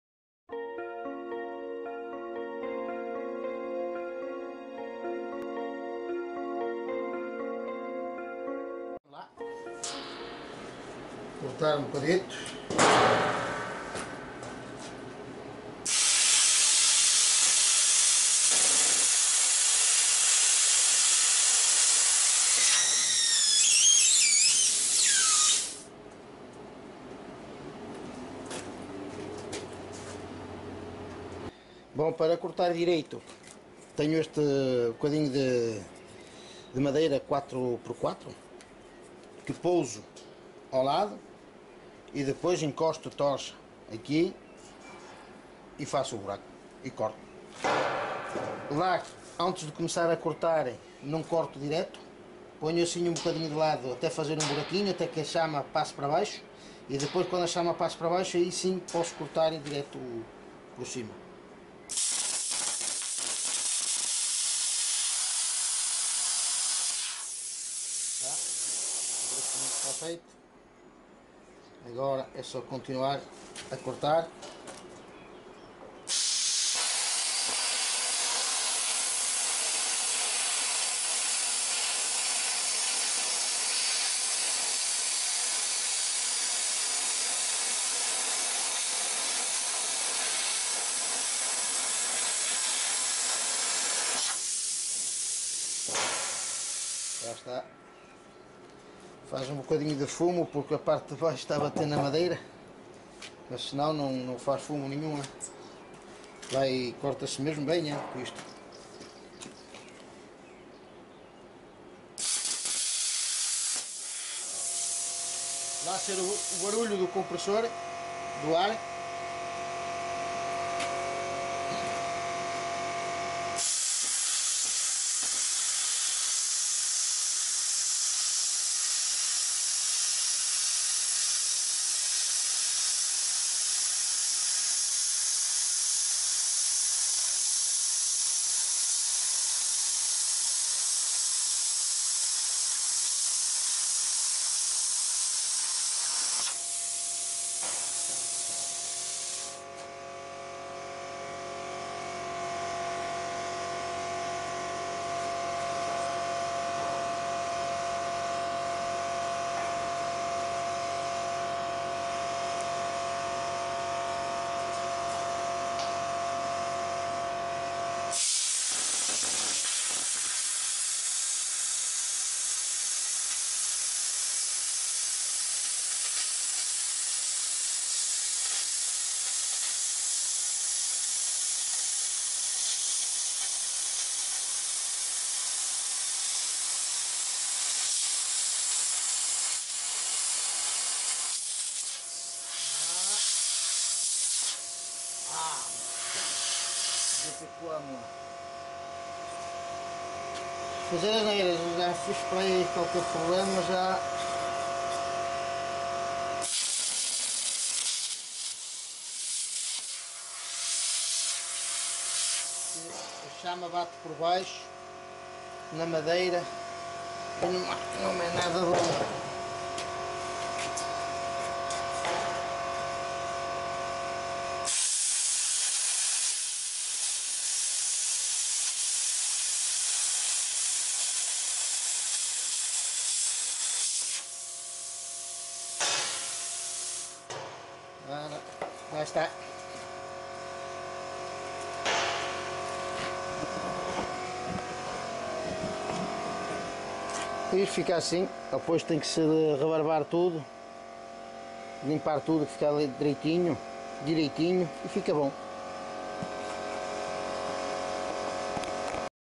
grade para proteçao da caixa das escadas feita na casa ( vidio 2)
cortar a chapa  com o  PLASMA  CUT 50